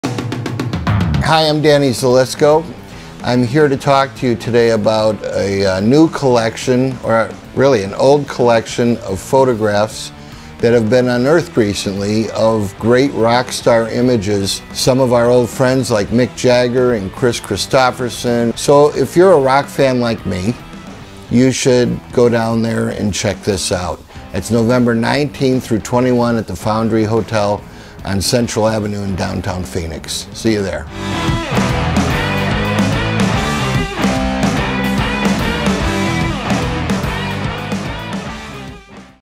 Rock & Roll Legends: The Lost Negatives of Michael Friedman, Phoenix 2021
Danny Zelisko promotes Phoenix Art Museum, November 2021 exhibit.